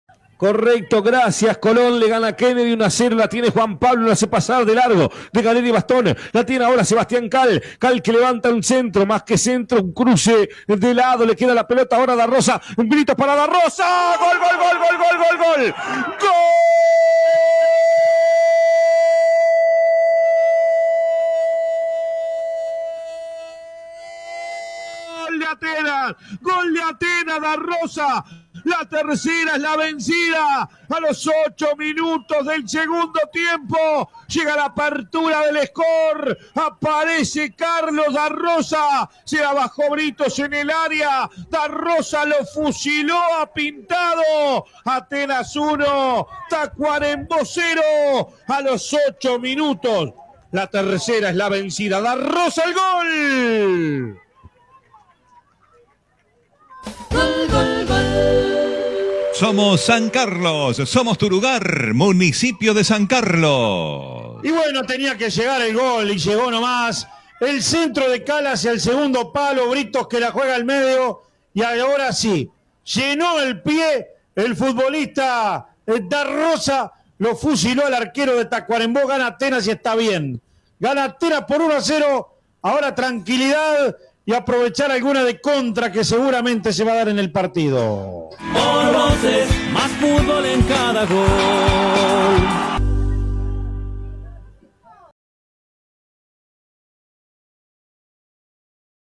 Atenas 1 Tacuarembó FC 0 - Segunda 2023 (por Paul Anderson)
Los goles del partido, por Paul Anderson y Víctor Hugo Castro para FM Voces de Maldonado.